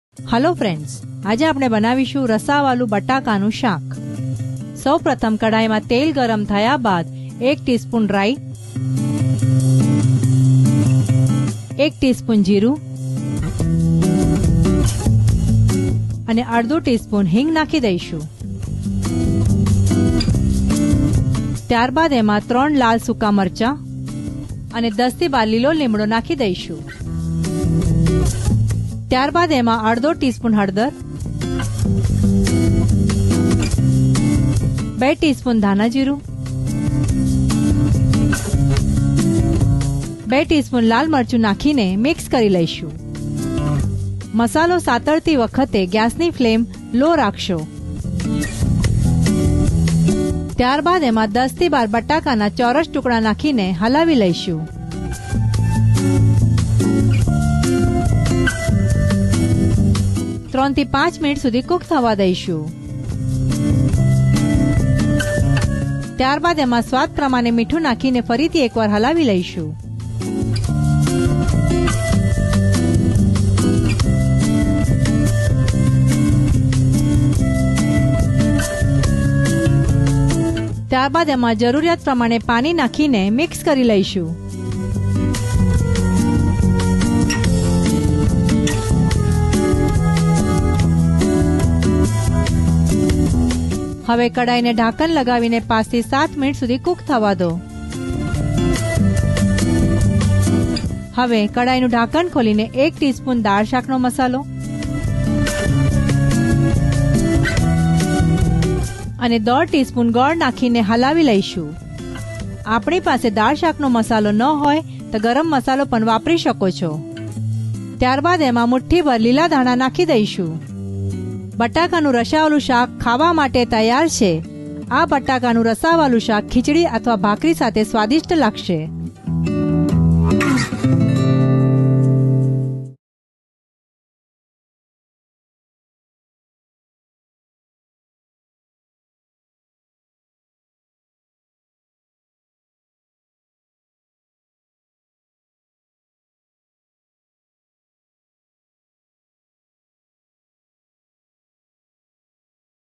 Bataka Nu Rassavalu Shaak | Recipes In Gujarati [ Gujarati Language] | Gujarati Rasoi
Bataka Nu Rassavalu Shaak is a traditional Gujarati recipe without which any Gujarati thali is incomplete. This recipe is a quick recipe to make and consists of cubed potatoes cooked in spicy gravy. The masala for the recipe consists of curry leaves, red chillies, mustard seeds and Gujarati Dal Shaak Masala. So do try out this traditional Gujarati recipe and let us know about the taste in the comment box.

Recipe:

Ingredients –
10-12 Cubed Potatoes
1 Teaspoon Mustard Seeds
1 Teaspoon Cumin Seeds
½ Teaspoon Hing
3-4 Dried Red Chillies
10-12 Curry Leaves
½ Teaspoon Turmeric Powder
2 Teaspoon Coriander-Cumin Seed Powder (Dhana Jeera)
2 Teaspoons Red Chilli Powder
½ Teaspoon Jaggery
1 Teaspoon Dal Shaak Masala
Salt
Water

Method –
1. Heat oil and add mustard seeds, cumin seeds,  hing, dried red chillies and curry leaves. Stir well.
2. Now, add turmeric powder, dhana-jeera powder, red chilli powder and mix well.
3. Add diced potatoes and mix well. Cook for 3-5 minutes.
4. Add salt as per taste.
5. Add water as per requirement and mix well.
6. Add Dal Shaak Masala and jaggery.
7. Top it with some coriander leaves and recipe is ready.